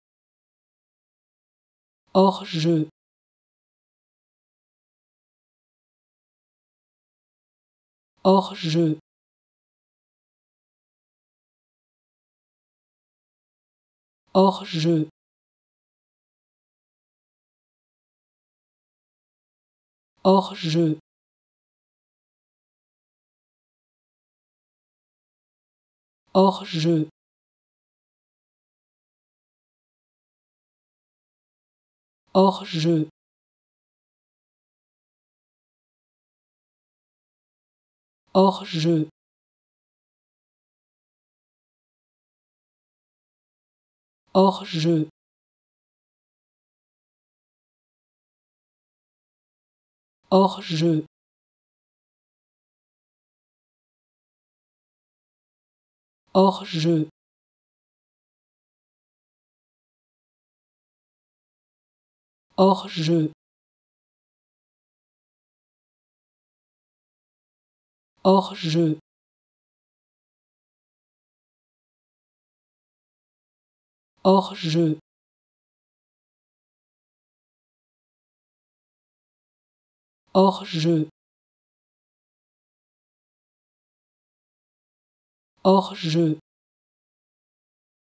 How to Pronounce ''Hors jeu'' (Off-side) Correctly in French
Learn how to say and properly pronounce ''Hors jeu'' in French with this free pronunciation tutorial.